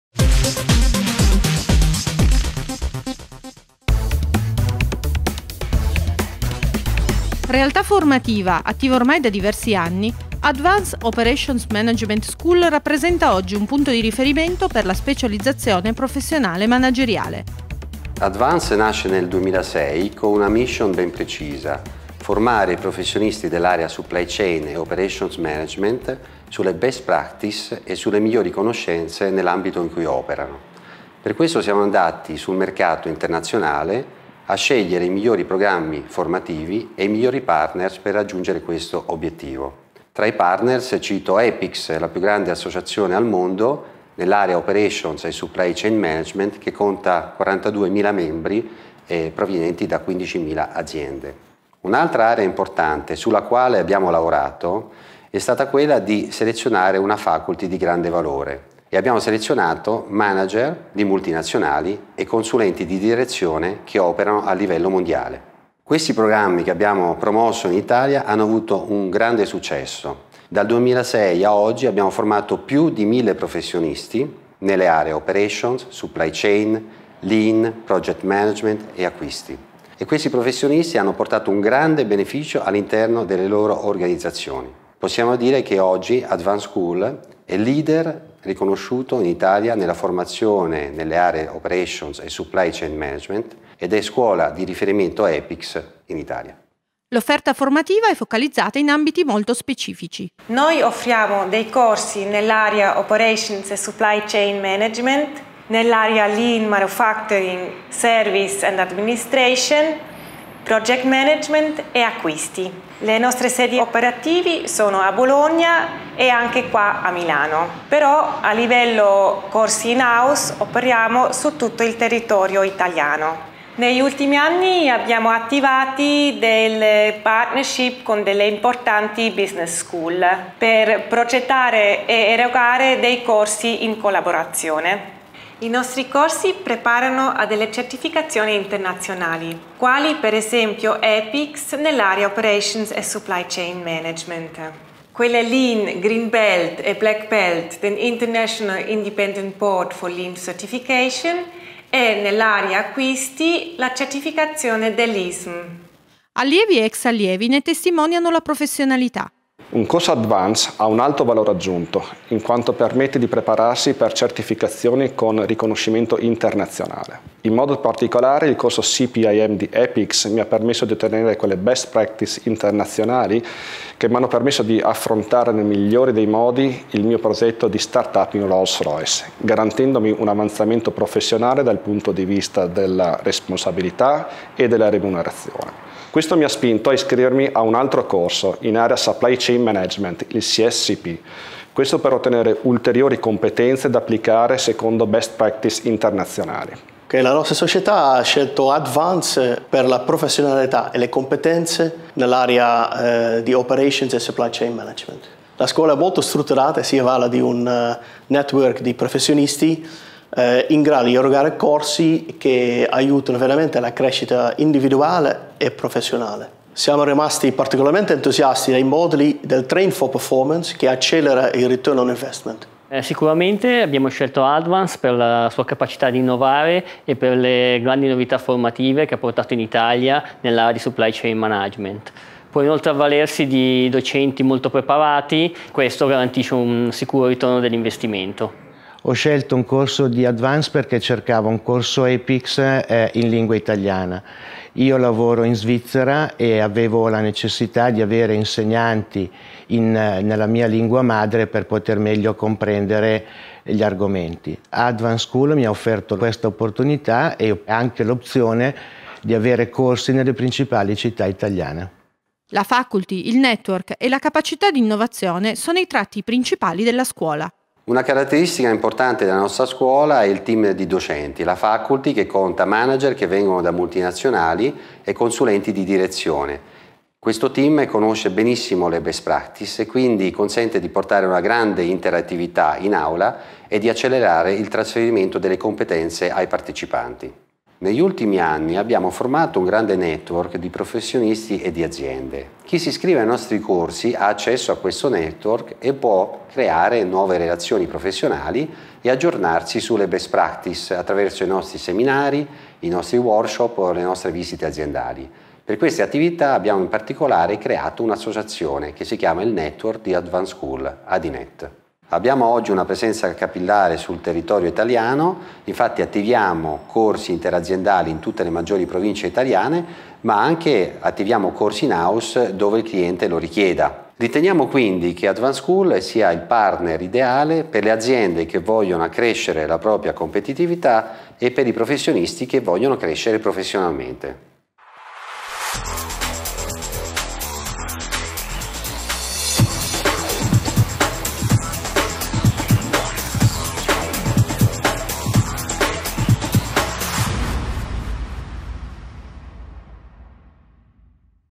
Alta formazione Advance Operations Management School
Advance Operations Management School è una scuola di formazione manageriale nell'area Operations e Supply Chain Management, Lean, Acquisti e Logistica.